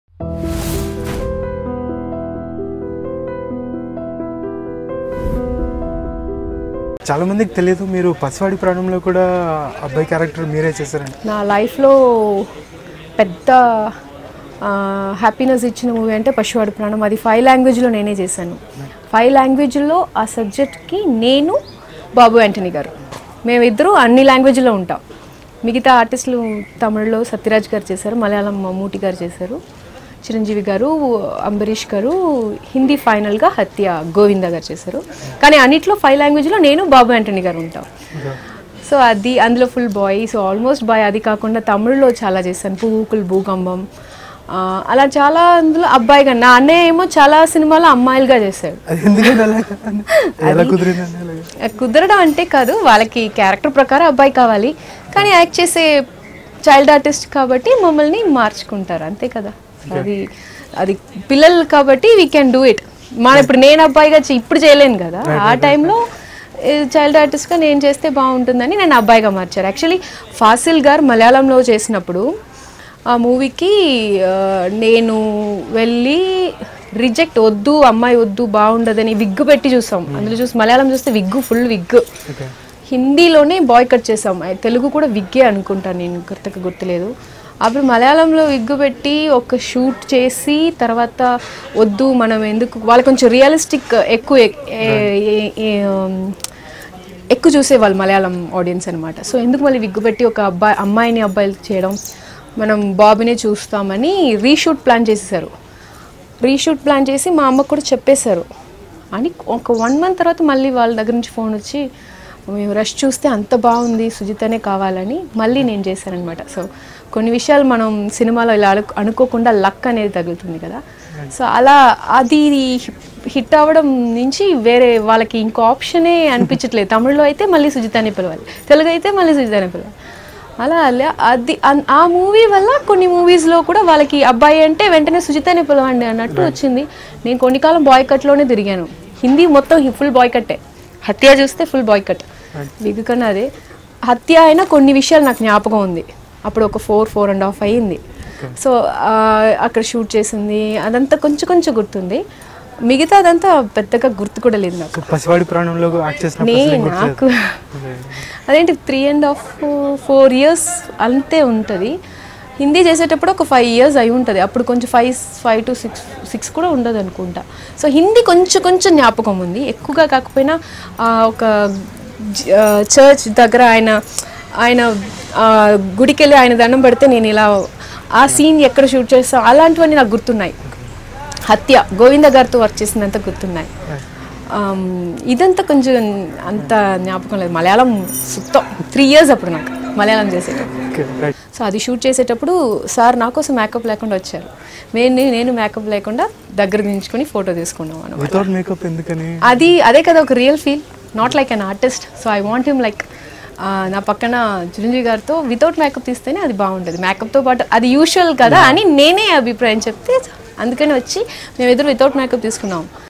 Pasivadi Pranam Movie Makes Me Happy - Actress Sujitha || Talking Movies with iDream
👉 Episode 1 A noted Television actress in Telugu, Tamil and Malayalam, Sujitha has been acting in films for more than three decades now. Her first tryst with camera began when she was as young as 41 days old in K Bhagyaraj's Mundhanai Mudichu and in Telugu, she shot to fame as the child artist in Chiranjeevi's Pasivadi Pranam (1987). Surprisingly, she went on to act in several Telugu and Tamil films as a boy, and later, she made her foray into Television, thanks to AVM Studios. Over the past two decades, she had acted with the likes of Chiranjeevi, Nagarjuna, Balakrishna among many others. Of late, although she has stayed away from films, Sujitha has made a comeback in Telugu with a new TV Serial, Aadavari Matalaku Ardhalu Verule, which is currently being aired on Gemini TV. Sujitha opens up about her life, working with several top actors and why she loves acting in Telugu TV serials.